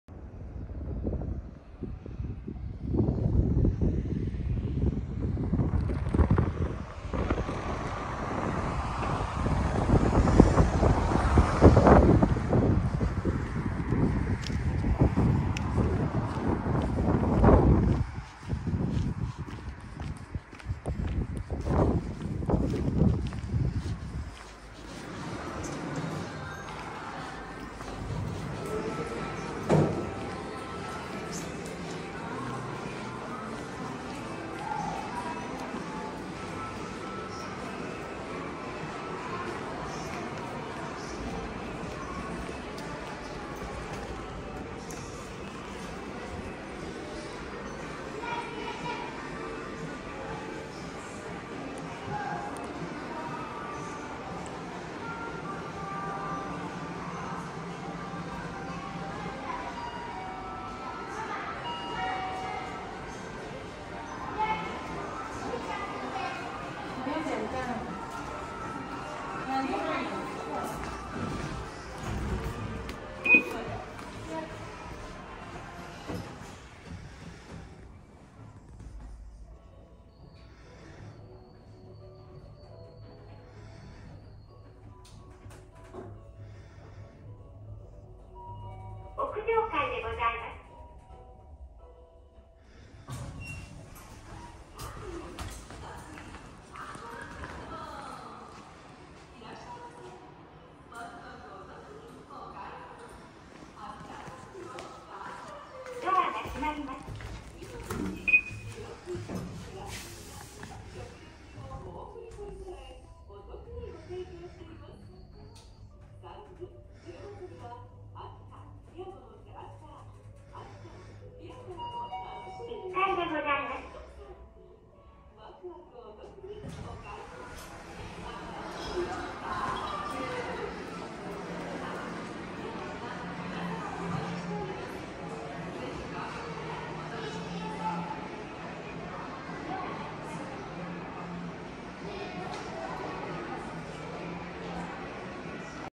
[アピタ伊賀上野店]日立製エレベーター 1F⇔Ｒ
やや古い日立製。

サムネのぬいぐるみはアピタのマスコットキャラクター『アピタン』